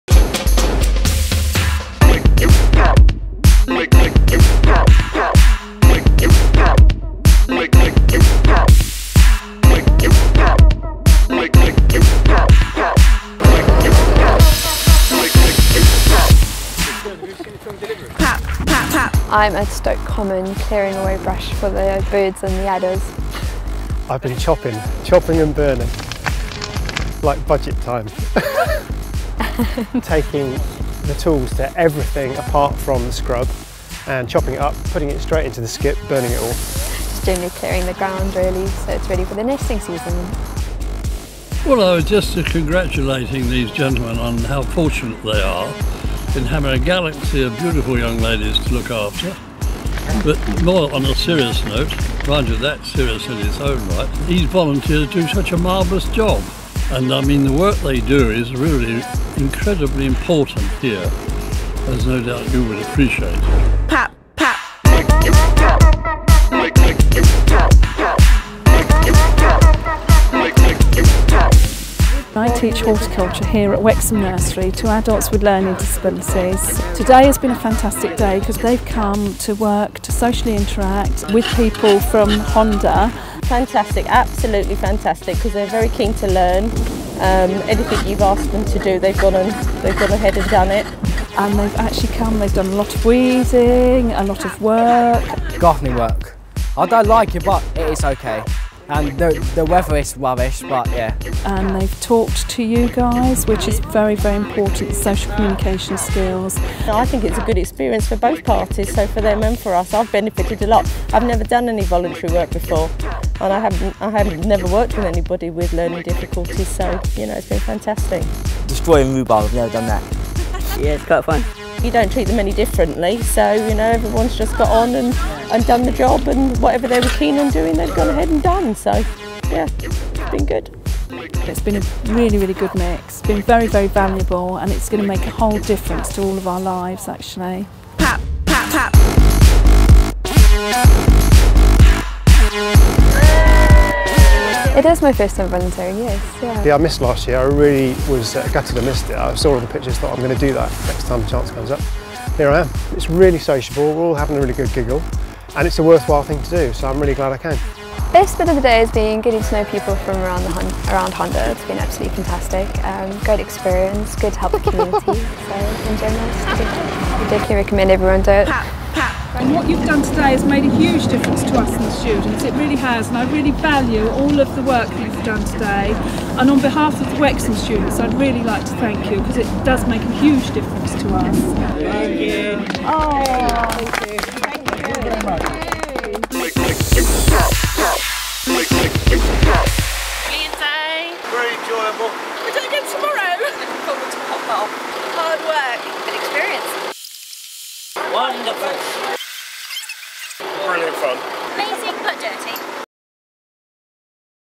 Honda head office employees' volunteer day
On 29 September 2010, over 40 employees from Honda Motor Europe's head office in Slough pitched in and gave a hand to two local community projects.

The two projects involved clearing Stoke Common's scrubland, and assisting adults with learning difficulties on gardening projects at Wexham Nursery.

Honda's philosophy has a strong focus on contributing to local communities and staff are encouraged to get involved as much as possible. This is the second year running that Honda has organised and taken part in these rewarding volunteering projects.

Jo Coaker, Events and Press Fleet Manager at Honda (UK), comments: "Our employees wanted to give back something back to their local community and what better way is there than to actually get out and spend a day volunteering in these very worthwhile initiatives. Last year many people found the day very rewarding and couldn't wait to get involved again".

Kirstin Bell, a Honda volunteer says of her day: "I had a fantastic day. It was hard work and although we got very muddy, it was a great experience and, as well as contributing to the community, we also bonded as colleagues and I made some good friends. I'm looking forward to next year's project already, but let's hope for better weather."